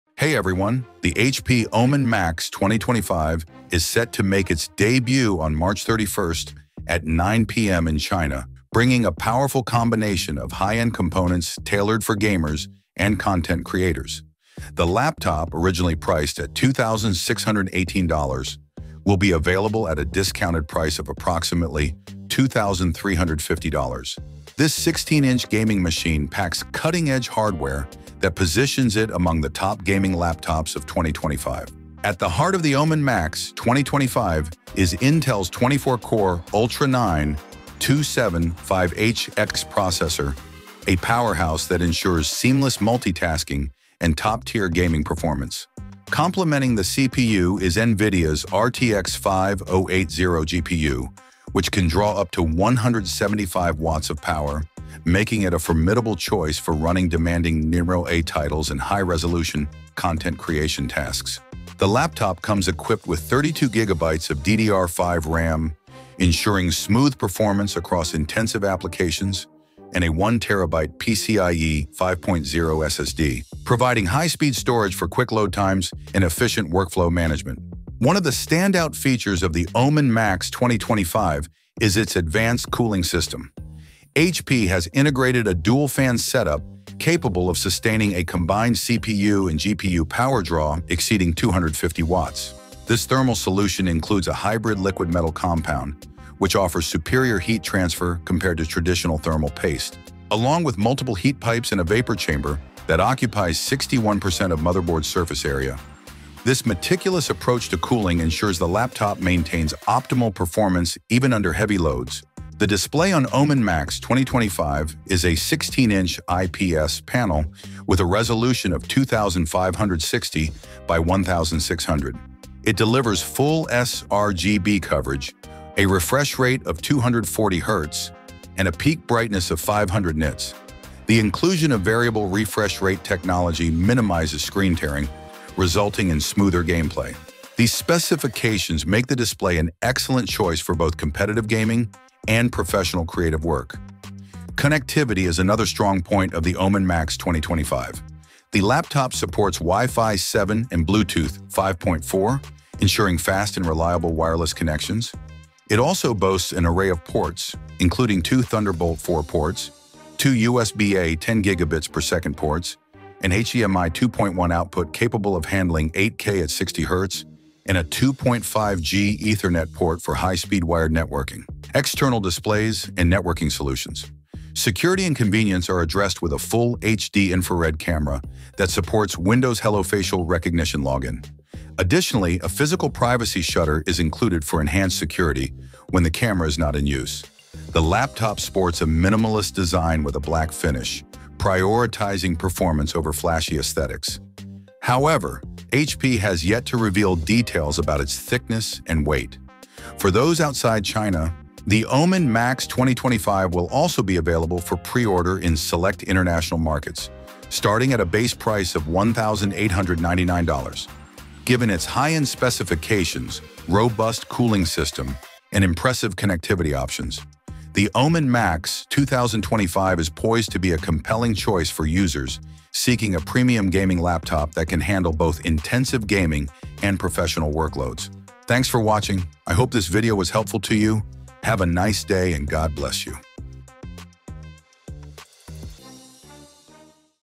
HP Omen MAX 2025 | The Most Powerful Gaming Laptop Yet?
HP’s Omen MAX 2025 launches March 31 with Intel Ultra 9-275HX (24-core) and RTX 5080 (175W TGP) – a $2,350 beast promising desktop-level gaming in a 16" chassis. With 32GB DDR5, PCIe 5.0 SSD, and hybrid liquid metal cooling, this might be 2025’s most extreme gaming laptop.

We analyze its 250W+ thermal performance, 4K gaming benchmarks, and content creation speed to see if it outshines the ASUS Scar 18. Can its dual-fan vapor chamber tame these specs without throttling?

Would you pay premium prices for this much power?